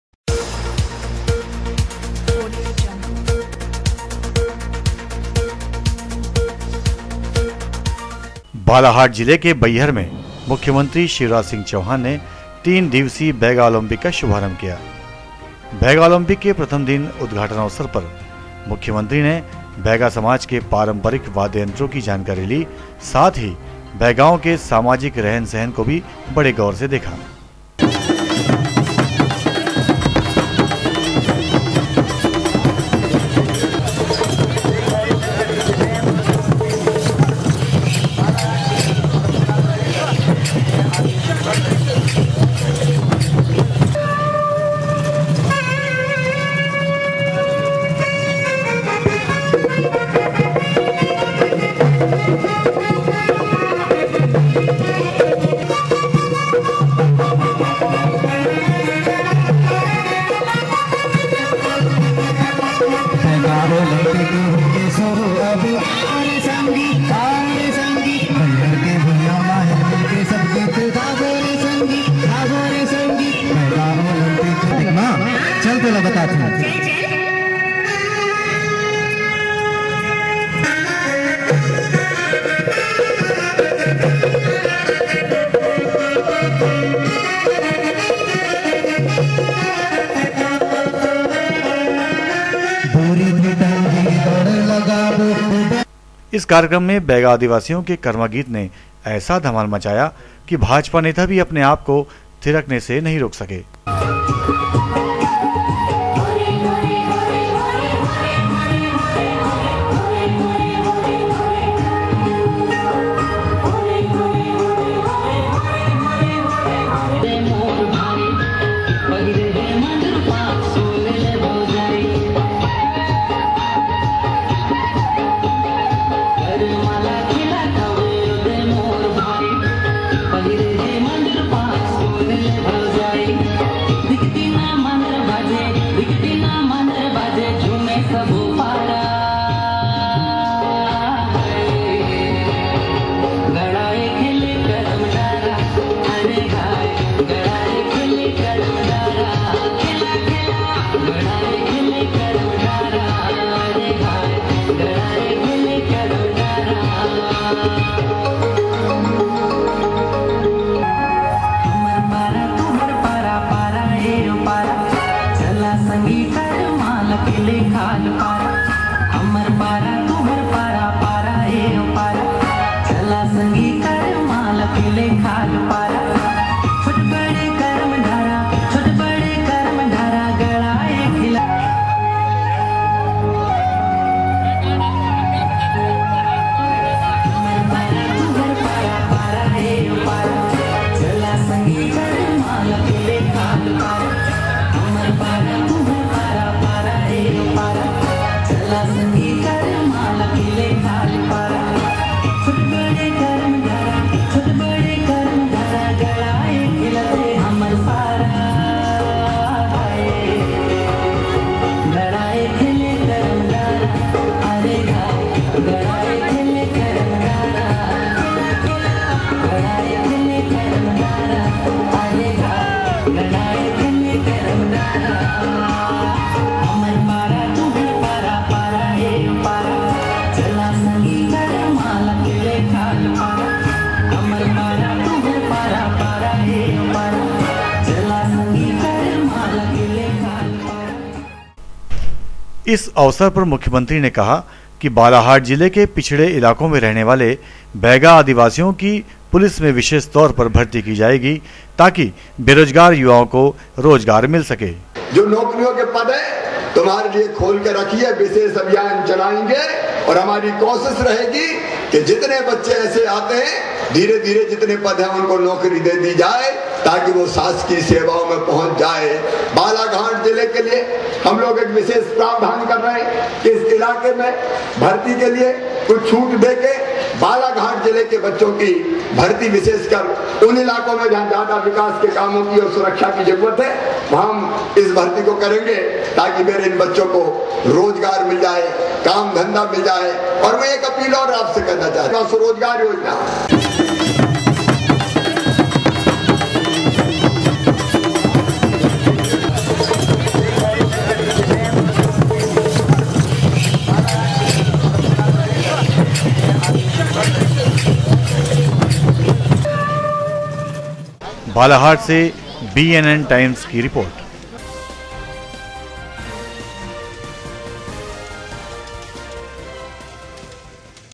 10 FEB 2017 CM BAIGA OLYMPIC BALAGHAT
बालाघाट जिले के बैहर में मुख्यमंत्री षिवराजसिंह चैहान ने 3 दिवसीय बैगा आॅलंपिक का शुभारंभ किया